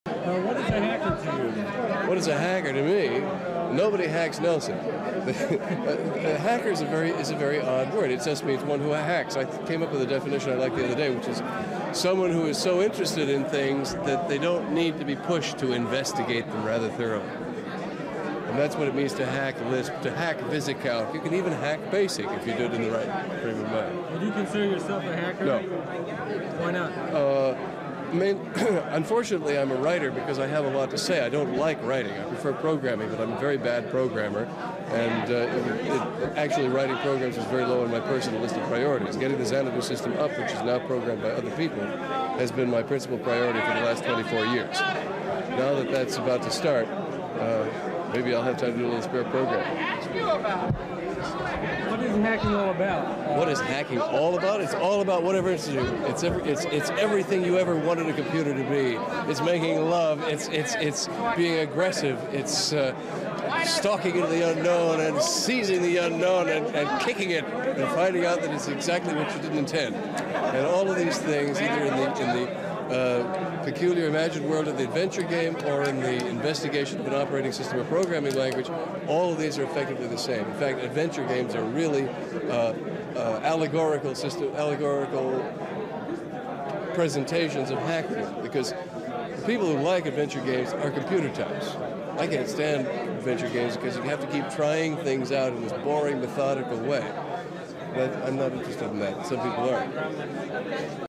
What is a Hacker? (first Hacker Conf, 1984)
From an interview after the first Hacker conference, 1984.